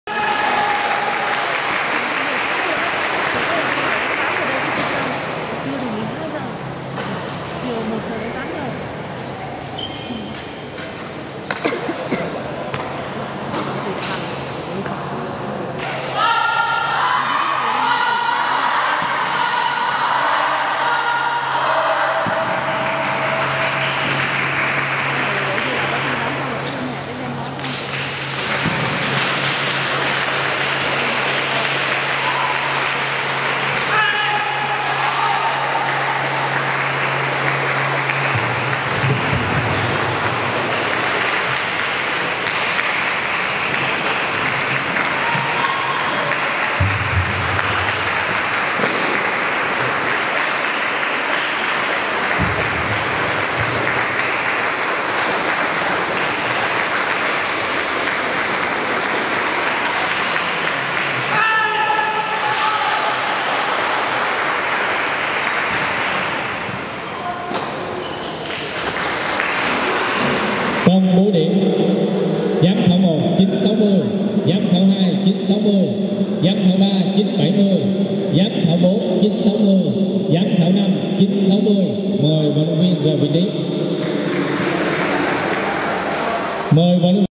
Lão Mai Quyền - Phan Trọng Hiếu - Giải Võ cổ truyền các tỉnh miền Đông Nam Bộ năm 2022
Giải võ cổ truyền các tỉnh miền Đông Nam Bộ năm 2022

Sáng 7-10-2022, tại Trung tâm huấn luyện và thi đấu thể dục thể thao tỉnh Bình Phước đã diễn ra lễ khai mạc Giải võ cổ truyền các tỉnh miền Đông Nam Bộ mở rộng, năm 2022, do Sở Văn hóa - Thể thao và Du lịch tỉnh Bình Phước đăng cai tổ chức.

Giải diễn ra từ ngày 7 đến 11-10, với sự tham gia của hơn 160 vận động viên. Các vận động viên đến từ các tỉnh, thành phố: Tây Ninh, Đồng Nai, Bình Dương, Bà Rịa - Vũng Tàu, Đắk Nông, Khánh Hòa và đoàn chủ nhà Bình Phước tham gia giải.

Giải võ cổ truyền các tỉnh miền Đông Nam Bộ mở rộng năm 2022 được tổ chức nhằm tạo điều kiện cho các đoàn giao lưu, thi đấu cọ xát, tăng cường hiểu biết lẫn nhau và nâng cao trình độ chuyên môn, chuẩn bị tốt nhất cho Đại hội Thể thao toàn quốc lần thứ IX diễn ra vào cuối năm nay.